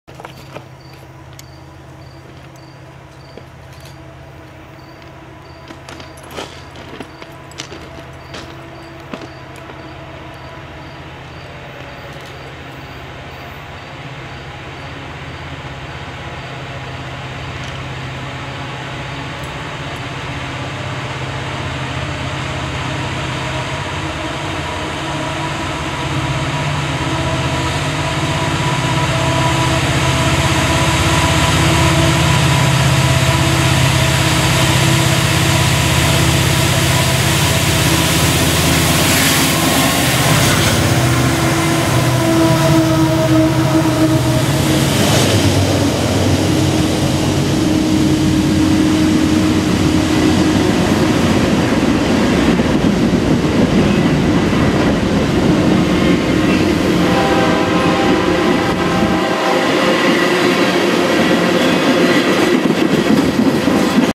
Thundering EMD's Charge Uphill
An older CN SD70M-2, SD75I, and C44-9W scream up the hill at Valparaiso on the old GTW.